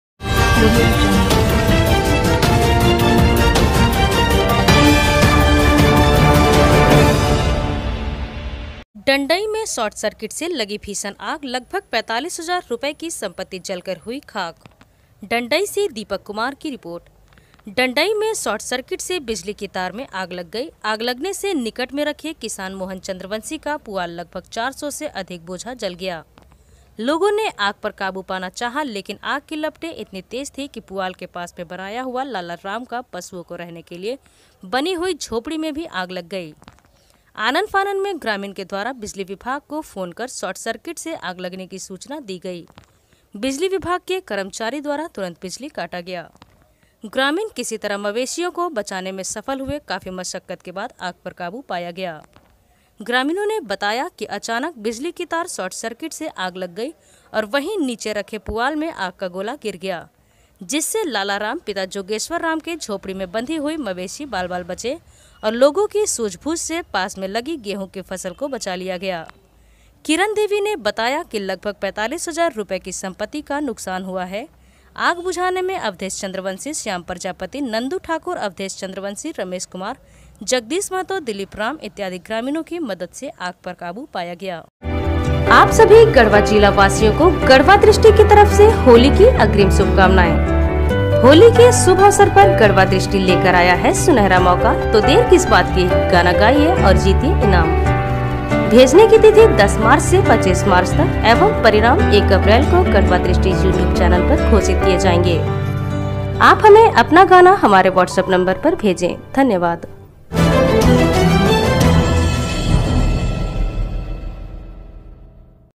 डंडई में शार्ट सर्किट से लगी भीषण आग : लगभग 45000 रुपए का संपत्ति जलकर हुआ खाक
डंडई से दीपक कुमार की रिपोर्ट

 डंडई में शॉटं सर्किट से  बिजली की तार में आग लग गई  आग लगने से निकट में रखें किसान मोहन चंद्रवंशी  का पुवाल लगभग 400 से अधिक  बोझा जल गया  लोगों ने आग पर काबू पाना चाहा लेकिन,आग की लपटे इतनी तेज थी पुआल के  पास में बनाया हुआ लाला  राम का  पशुओं को  रहने के लिए  बना हुआ झोपड़ी में भी आग लग गई।  आनन फानन में ग्रामीण के द्वारा बिजली विभाग को फोन कर शार्ट सर्किट से आग लगने की सूचना दी गई । बिजली विभाग के कर्मचारी द्वारा तुरंत बिजली काटा गया।
ग्रामीण किसी तरह मवेशी को बचाने में सफल हुए । काफी मशक्कत के बाद आग पर काबू पाया गया
ग्रामीणों ने बताया अचानक बिजली की तार शॉर्ट सर्किट से  आग लग गई और वही नीचे रखे पुवाल मे आग का गोला गिर गया जिससे  लालाराम पिता जागेश्वर राम के झोपड़ी में बंधे हुए मवेशी बाल बाल बचे और
लोगों की सूझ-बूझ से पास में लगी गेहूं की फसल को  बचा लिया गया ।
किरण देवी ने बताया कि लगभग  45000 रुपए के संपत्ति का नुकसान हो गया । आग बुझाने में अवधेश  चंद्रवंशी , श्याम प्रजापति  नंदू ठाकुर ,अवधेश चंद्रवंशी ,रमेश कुमार ,जगदीश महतों, दिलीप राम इत्यादि ग्रामीणों की मदद से आग पर काबू पाया गया